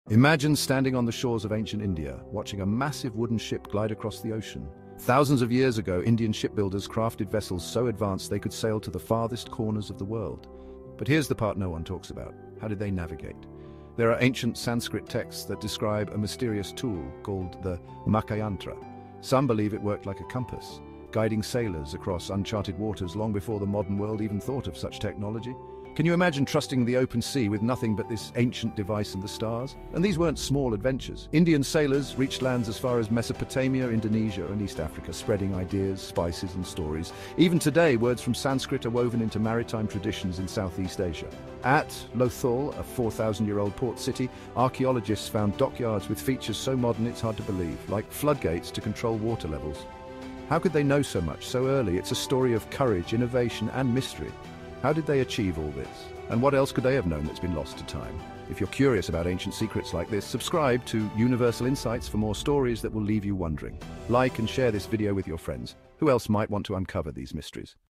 How INDIAN Sailors Ruled the Seas! 4,000-Year-Old Indian Shipbuilding Genius
How did ancient India dominate the seas thousands of years ago? This video uncovers the fascinating story of Indian shipbuilders, sailors, and navigators who mastered maritime trade and technology long before the modern era. Discover the mysterious macchayantra, an ancient compass-like tool, and the incredible engineering of Lothal’s 4,000-year-old dockyards. Learn how Indian sailors connected continents, spread knowledge, and built one of the greatest maritime legacies in history.

Questions answered in this video:
How did ancient India excel in shipbuilding and navigation?
What was the macchayantra? Did ancient Indians use a compass?
What evidence do we have of ancient Indian maritime trade?
How did Indian sailors travel to distant lands like Mesopotamia and Africa?
What secrets do ancient ports like Lothal reveal about Indian engineering?

Bibliography & References:
Rao, Shikaripura Ranganatha. Lothal and the Indus Civilization.
McIntosh, Jane. The Ancient Indus Valley: New Perspectives.
Tripathi, Vibha. History of Ancient India: Ports and Maritime Trade.
Balakrishna, M. R. Maritime Heritage of India.
Publications from the Archaeological Survey of India on Lothal excavations.
Research papers on macchayantra from the Indian Journal of History of Science.
National Maritime Foundation articles on India’s ancient seafaring history.